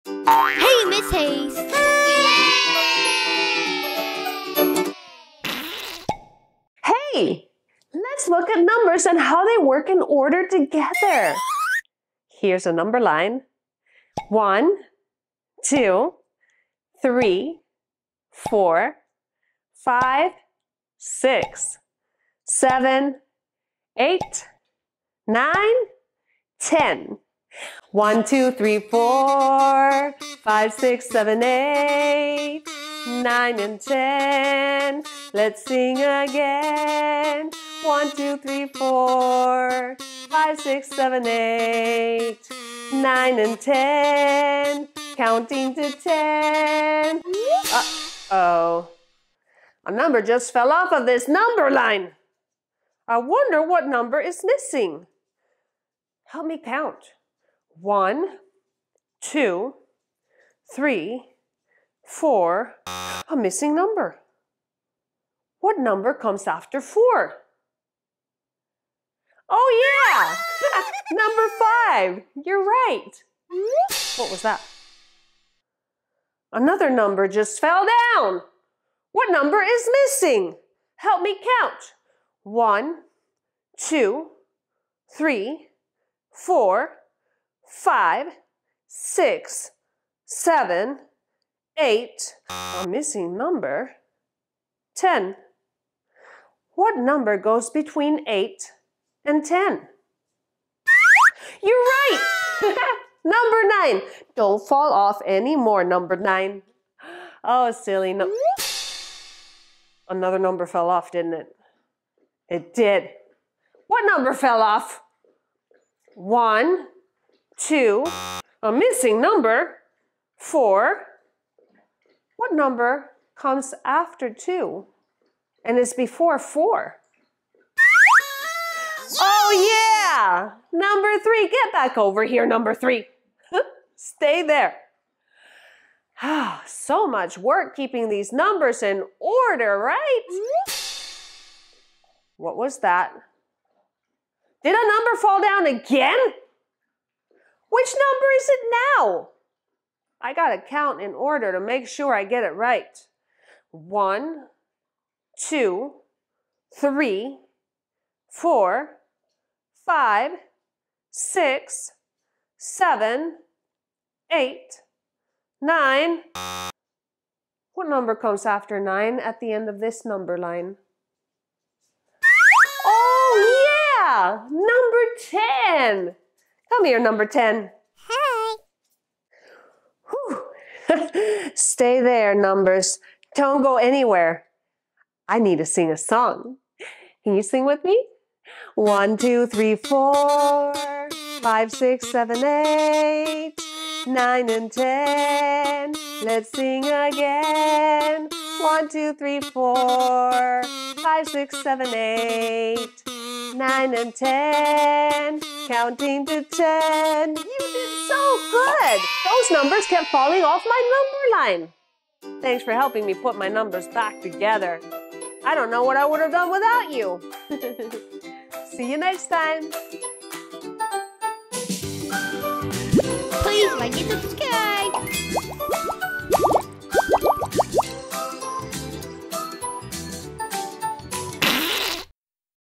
sing and count- with a number line!
Come with me for a super fun sing-along adventure on the number line!

-Oh no!

The numbers are falling off!

I need your help to put them back in the right order. Let's sing and learn together as we figure out what numbers are missing.

Perfect for toddlers and preschoolers learning number order and counting. Get ready to sing, dance, and learn with Mrs. Hays!"